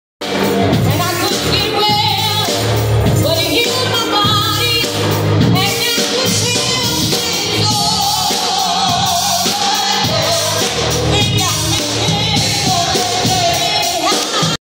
Choir at the house of worship. Jacksonville, IL. praise Jesus / God.
The house of worship choir singing.

  Praise Jesus/ God